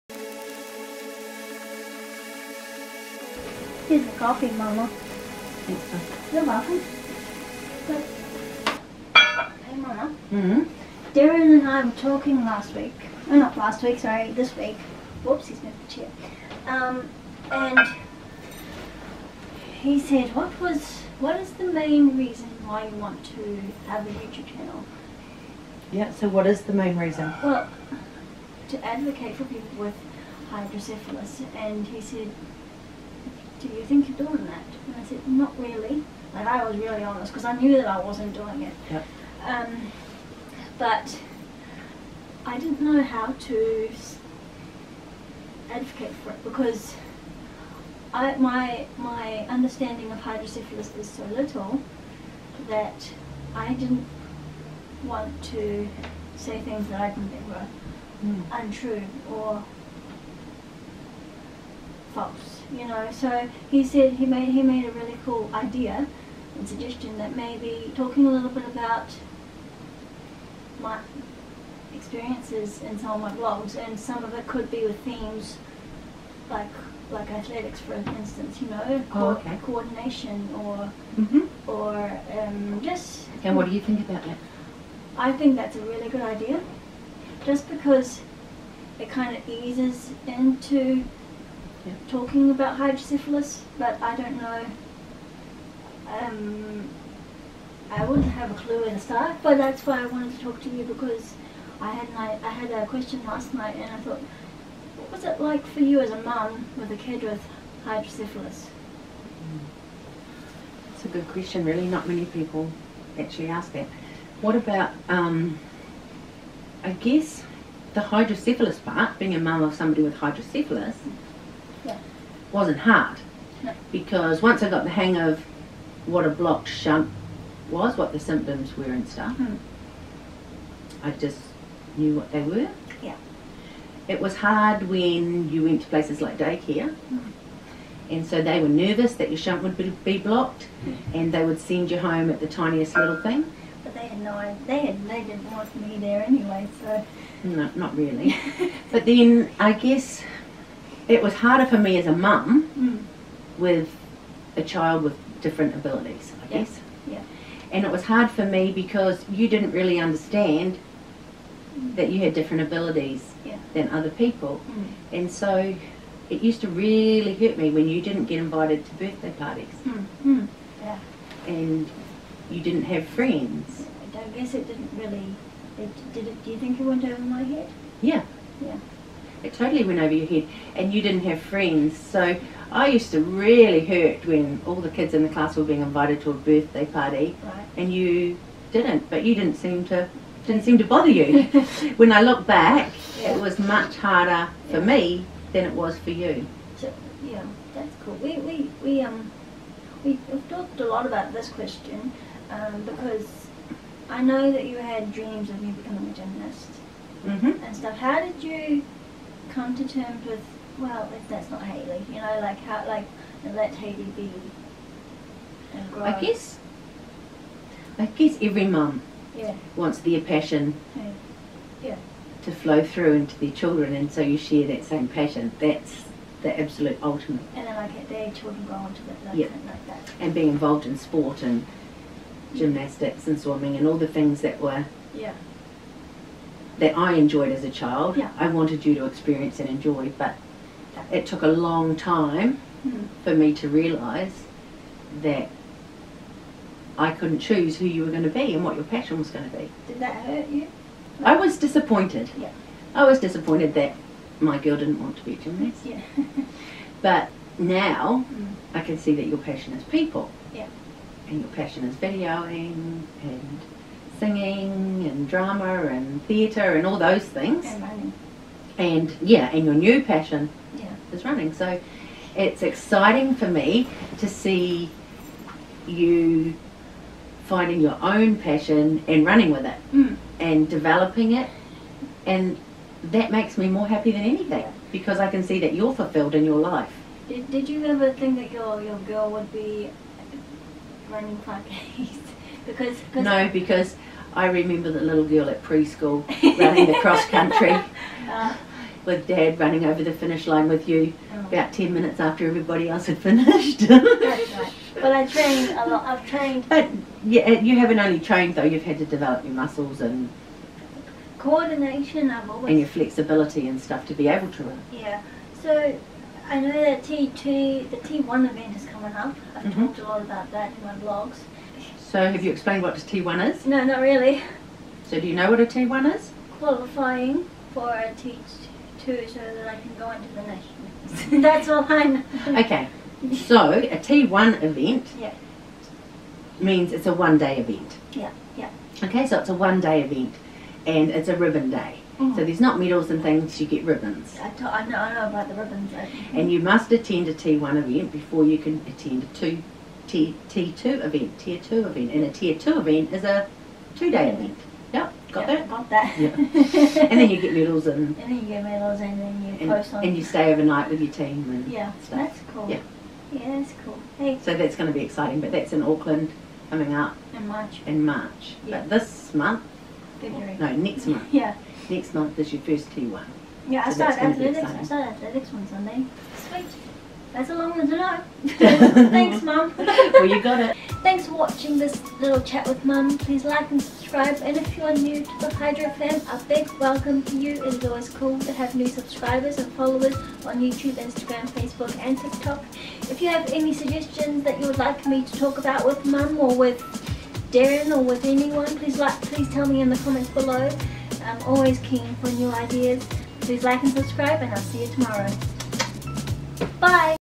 Coffee with Mumma (pilot) l Hydro Hayley
Welcome to a new series I'm calling "Coffee with Mumma"! This is going to a great series were my Mumma and I talk about things that she and I both struggled with and things that we both achieved as I was growing up. Side note this was filmed ages ago. Didn't think I would post it. Hope you enjoy this cool video!

"Every day's a challenge, so let's embrace it!"